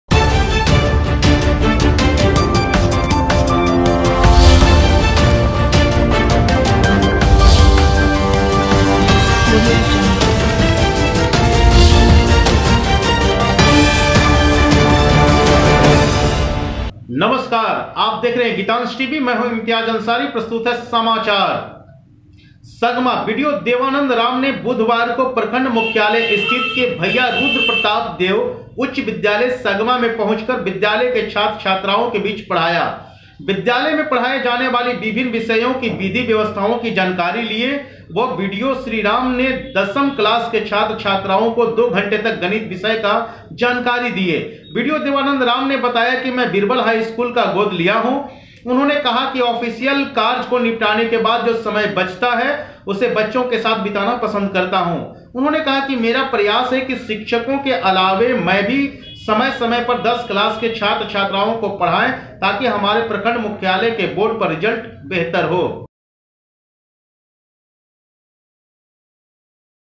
बी डी ओ देवानन्द राम द्वारा दी गयी गणित विषय की जानकारी
संवददाता:सगमा से राजुरंजन की रिपोर्ट

सगमा :  बीडीओ देवानंद राम ने बुधवार को प्रखंड मुख्यालय स्थित मे भैया रूद्र प्रताप देव उच्च विद्यालय सगमा मे पहुंच कर विद्यालय के छात्र छत्राए के बीच पढ़ाया । विद्यालय मे पढाने जाने वाली विभिन्न विषयो के विधी व्यवस्थाओ का जानकारी लिए व बीडीओ श्री राम ने दशम कलाश के छात्र छत्राओ को दो घंटे तक गणित विषय का जानकारी दिए बिडीओ देवानंद राम ने बताया कि मै बिरवल हाई स्कूल का गोद लिया हु  । उन्होंने ने कहा कि ऑफिसियल कार्य को निपटाने के बाद जो समय बचता है उसे बच्चे के साथ बिताना पसंद करता हु । उन्होंने कहा कि मेरा प्रयास है कि शिक्षको के अलावे मै भी समय समय पर दश कलाश के छात्र छत्राओ को पढाए तकि हमारे प्रखंड मुख्यालय के बोर्ड का रिजल्ट बेहतर हो ।